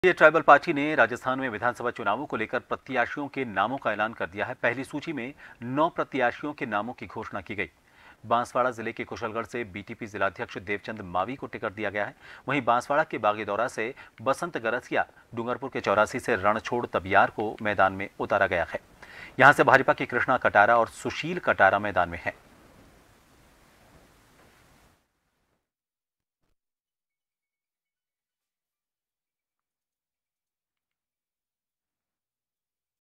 Banswara News | विधानसभा चुनाव के लिए BTP ने उतारे प्रत्याशी, 9 सीटों पर उम्मीदवार किये घोषित
Watch JAN TV on :
Tata Play DTH : 1185
Airtel DTH: 355
JIO Fiber: 1384

News Credit-JST